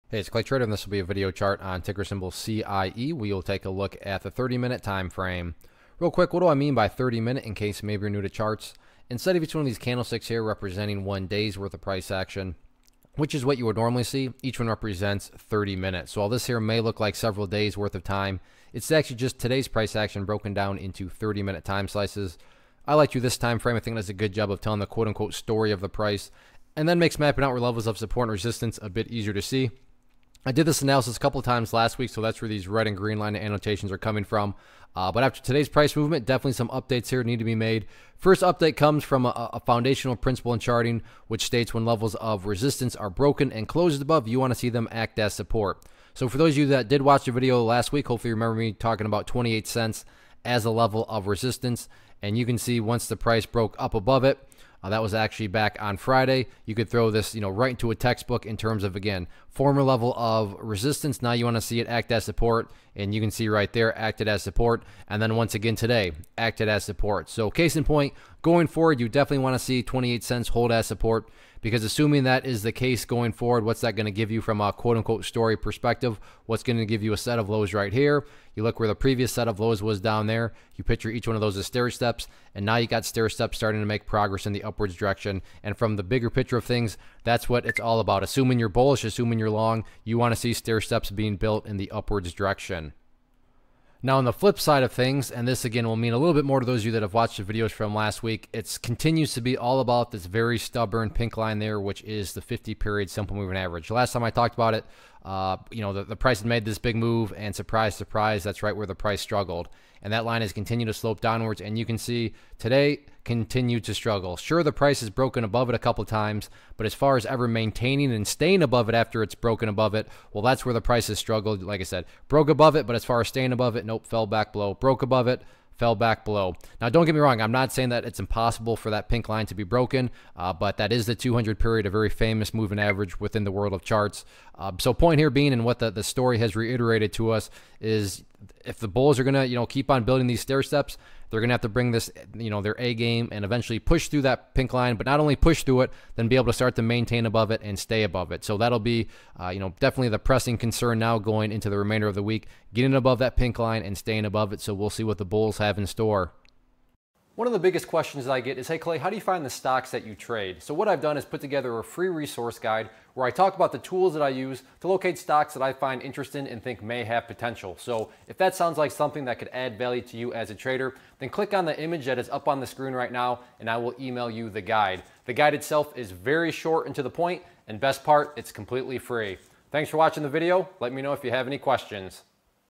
Cobalt International Energy - CIE Stock Chart Technical Analysis for 05-22-17
Learn how to read stock charts and identify technical patterns as ClayTrader does a quick stock chart review on Cobalt International Energy (CIE).